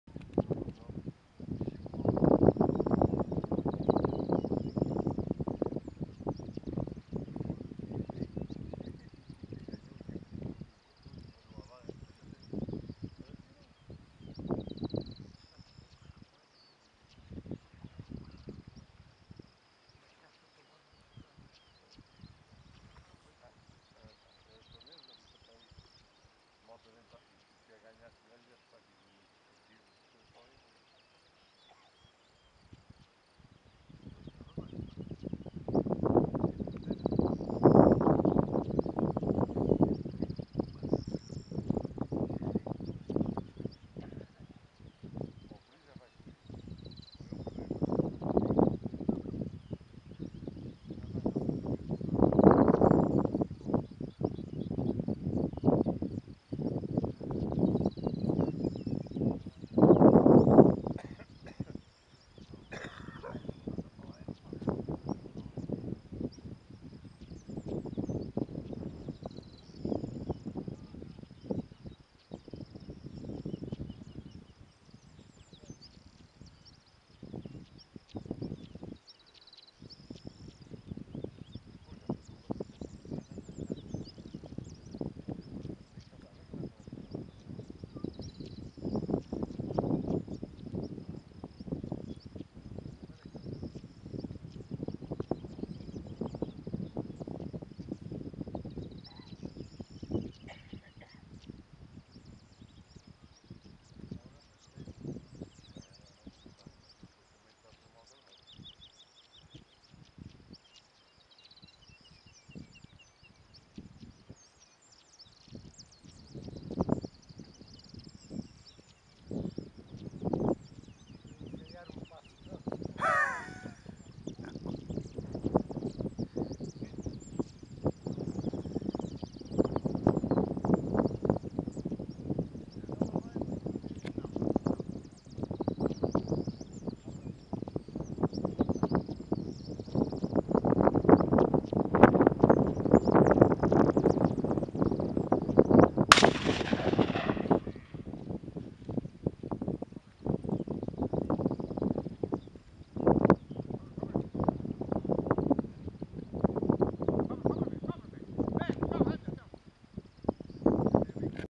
Santo Huberto Cadaval
Carlos Guilherme com Epagneul Breton de nome Beck.